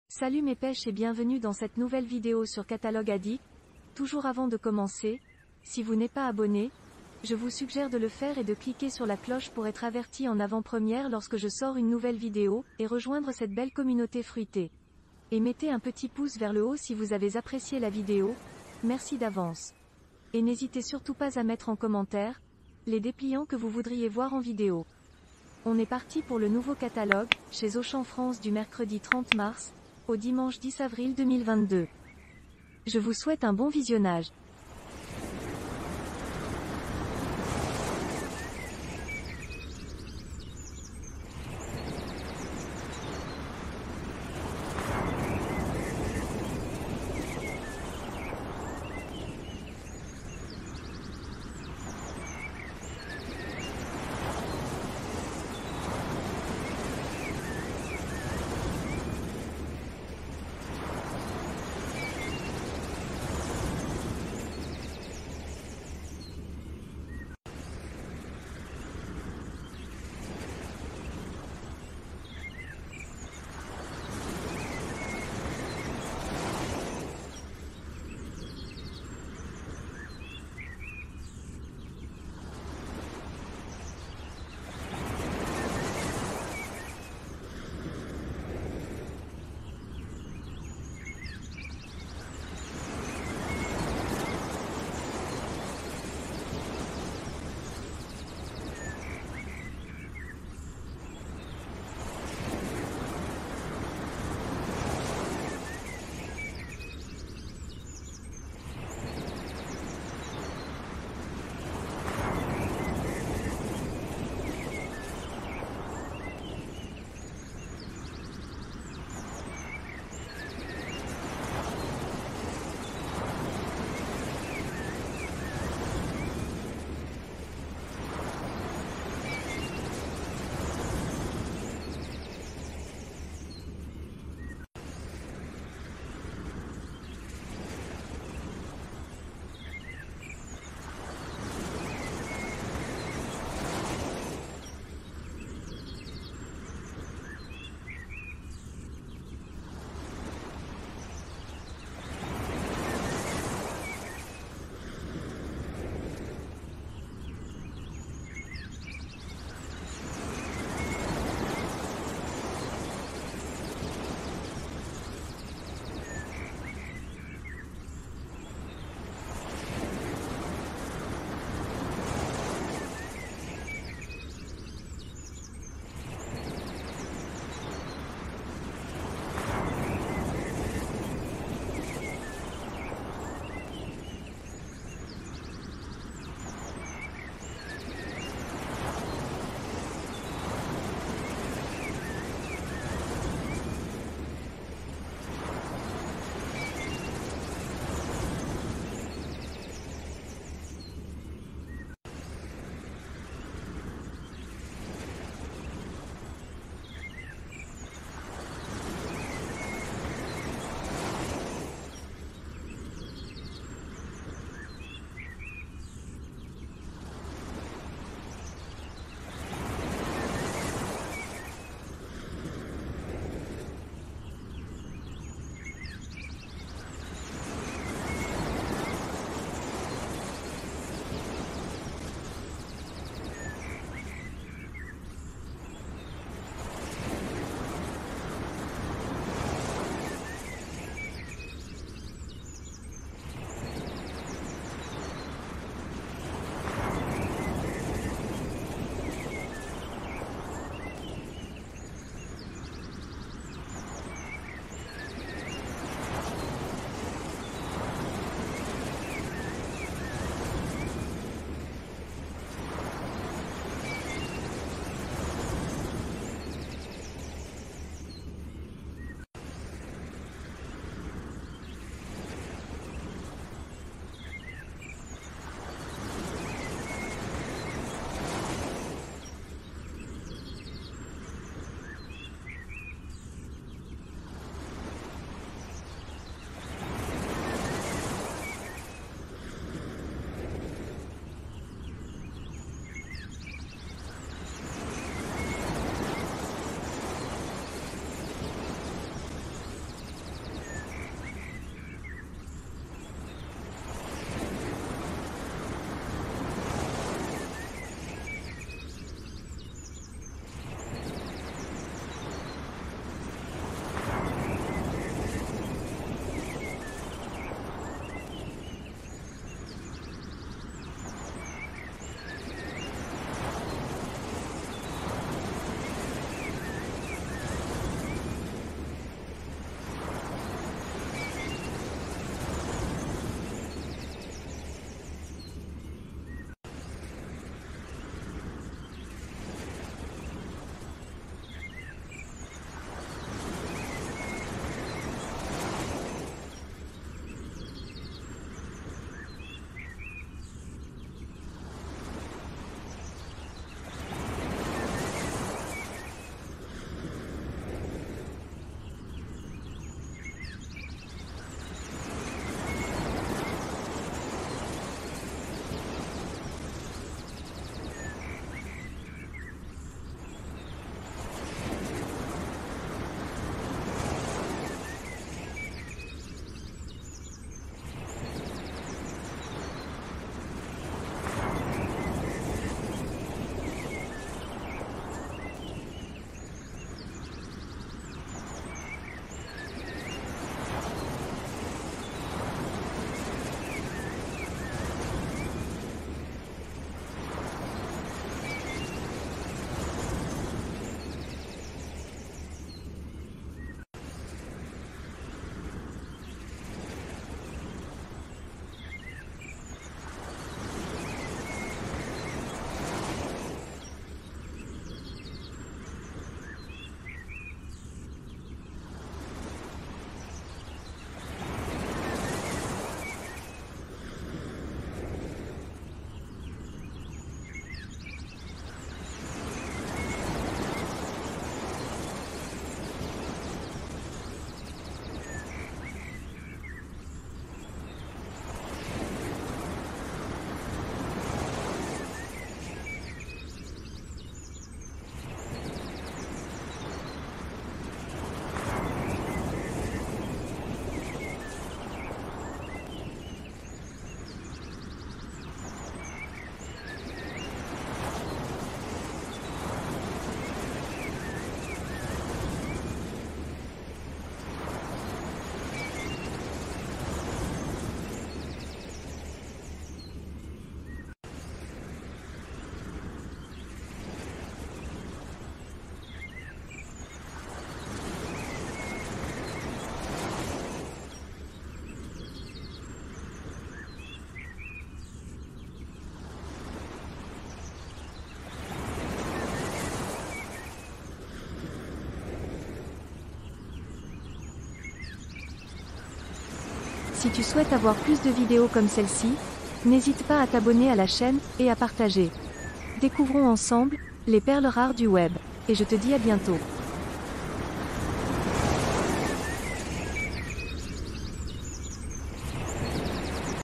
catalogue AUCHAN du 30 mars au 10 avril 2022 💛 Arrivage - FRANCE
🛒🛒NOUVEAU catalogue chez 📍AUCHAN📍 du mercredi 30 mars au dimanche 10 avril 2022 en FRANCE!